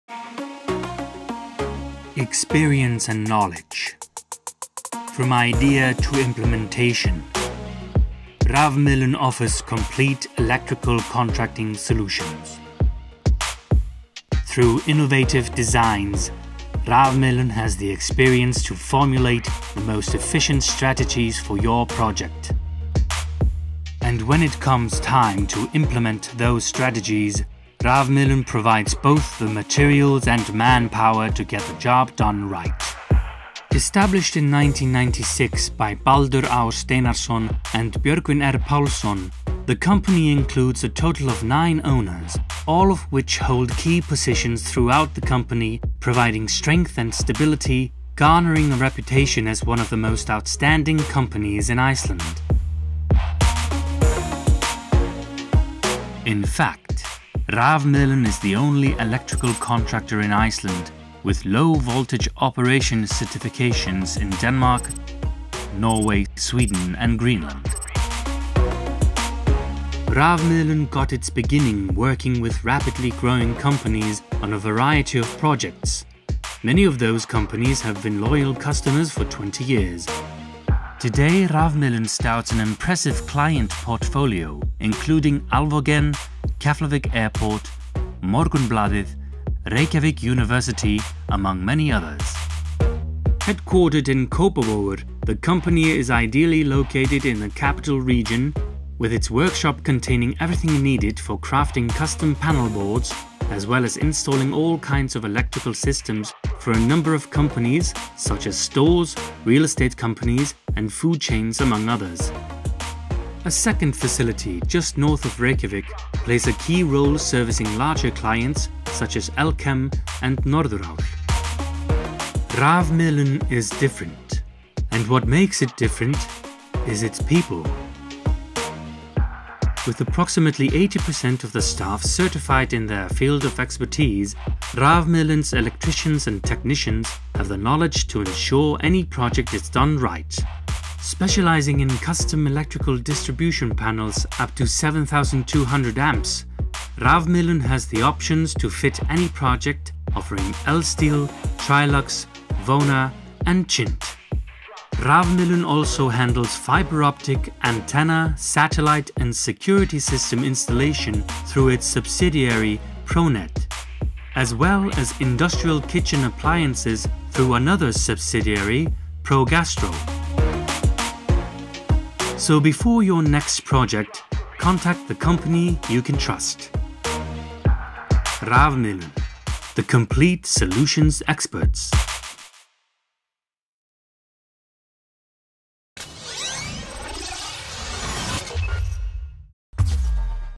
Rafmiðlun - English
Experience and knowledge.

From idea to implementation, Rafmiðlun offers complete electrical contracting solutions.

Through innovative designs, Rafmiðlun has the experience to formulate the most efficient strategies for your project.

And when it comes time to implement those strategies, Rafmiðlun provides both the materials, and manpower to get the job done right.

Established in 1996 by Baldur Á. Steinarsson, and Björgvin R. Pálsson, the company includes a total of nine owners, all of which hold key positions throughout the company, providing strength and stability, garnering a reputation as one of the most outstanding companies in Iceland.

In fact, Rafmiðlun is the only electrical contractor in Iceland with low voltage operation certifications in Denmark, Norway, Sweden, and Greenland.

Rafmiðlun got its beginning working with rapidly growing companies on a variety of projects. Many of those companies have been loyal customers for 20 years.

Today Rafmiðlun stouts an impressive client portfolio, including Alvogen, Keflavík airport, Morgunblaðið, Reykjavík University, among many others.

Headquartered in Kópavogur, the company is ideally located in the capital region, with its workshop containing everything needed for crafting custom panel boards, as well as installing all kinds of electrical systems for a number of companies, such as stores, real estate companies, and food chains among others.

A second facility, just north of Reykjavik, plays a key role servicing larger clients, such as Elkem and Norðurál.

Rafmiðlun is different. And what makes it different is its people.

With approximately 80% of the staff certified in their field of expertise,  Rafmiðlun’s electricians and technicians have the knowledge to ensure any project is done right.

Specializing in custom electrical distribution panels up to 7200 amps,  Rafmiðlun has the options to fit any project, offering ELSTEEL, Trilux, Wöhner, and Chint.

Rafmiðlun also handles fiber optic, antenna, satellite, and security system installation through its subsidiary ProNet, as well as industrial kitchen appliances through another subsidiary, ProGastro.

So before your next project, contact the company you can trust.

Rafmiðlun...the complete solutions experts.